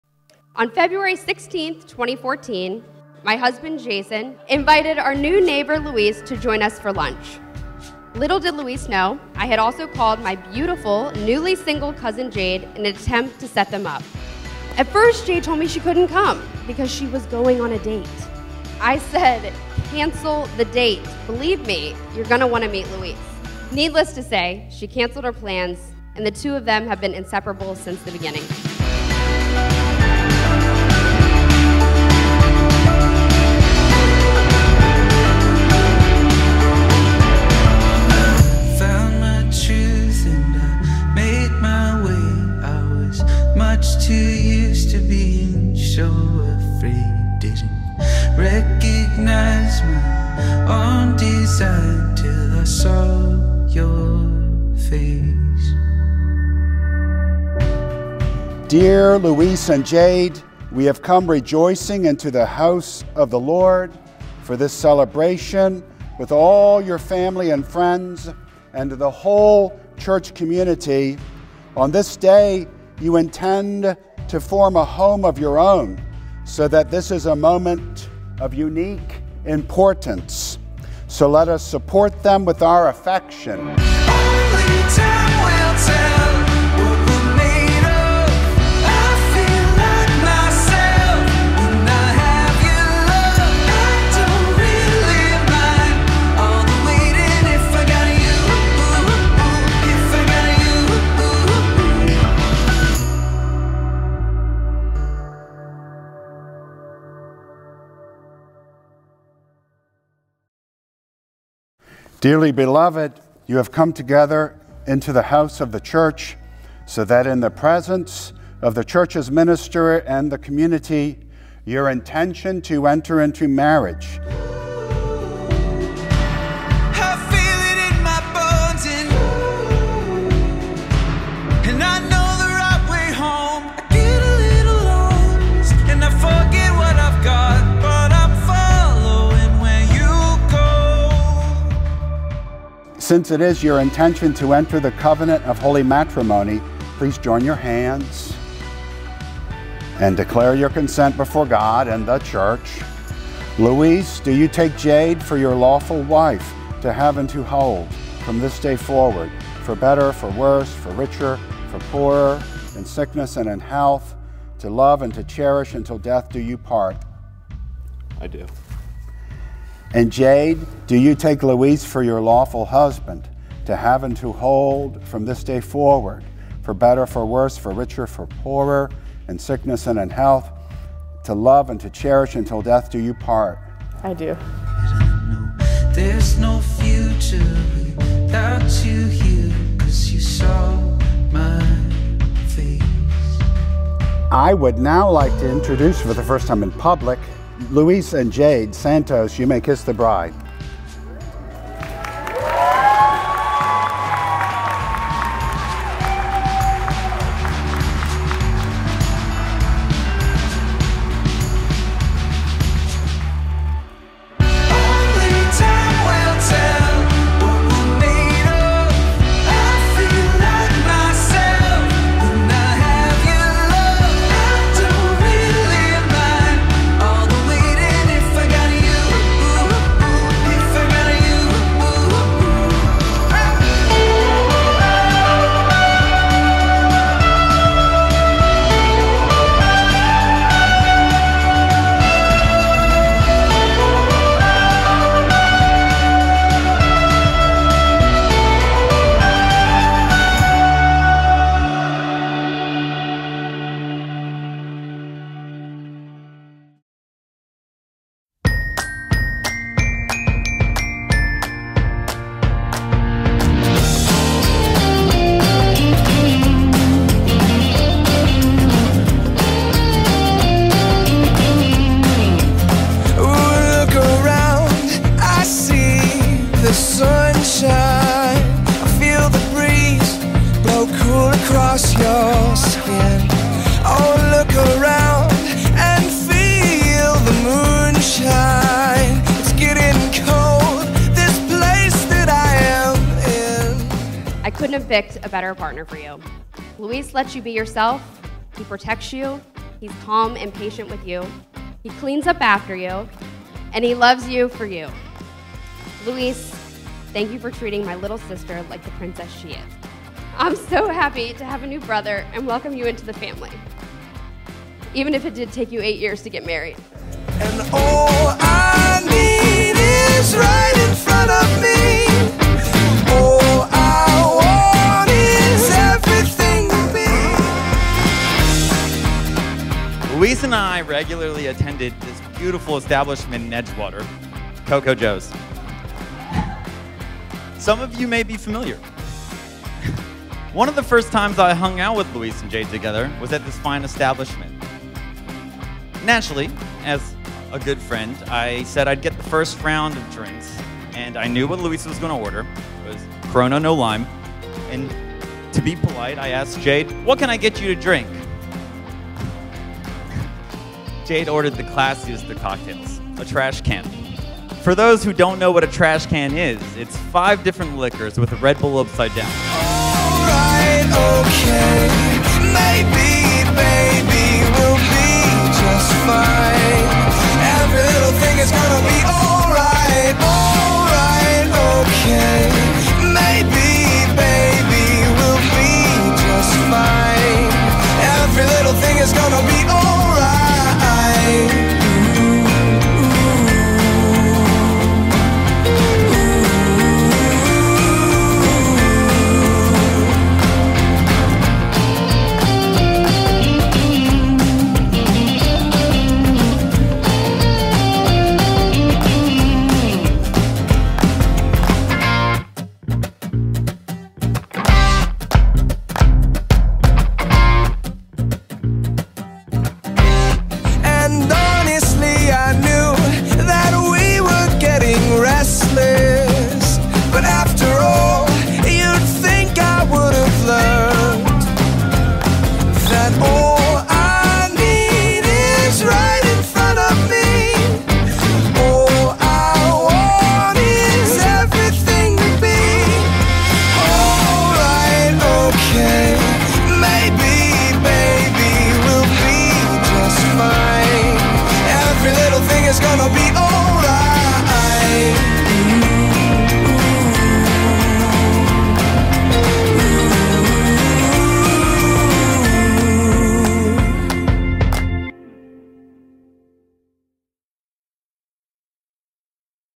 "Jade ordered the classiest of cocktails; a Trash Can" An Anchor Inn Wedding Video
The Full Highlight of Jade & Luis' beautiful wedding day at the Anchor Inn in Pasadena, MD!